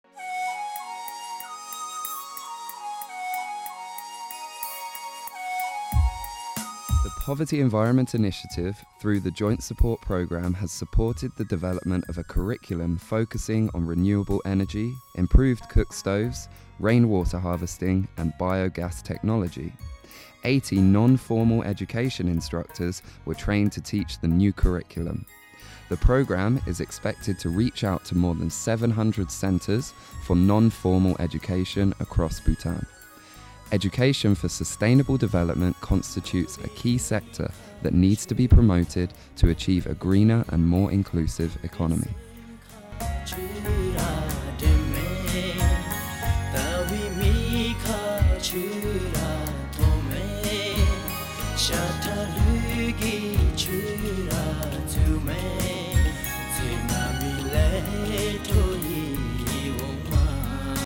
Education for a Greener, inclusive Economy in Bhutan
The Poverty-Environment Initiative, through the Joint Support Programme has supported the development of a curriculum focusing on renewable energy, improved cook stoves, rain water harvesting and bio-gas technology. 80 Non-Formal Education Instructors were trained to teach the new curriculum. The programme is expected to reach out more than 700 centers for non-formal education across Bhutan. Education for Sustainable Development constitutes a key sector that needs to be promoted to achieve a Greener and more inclusive Economy.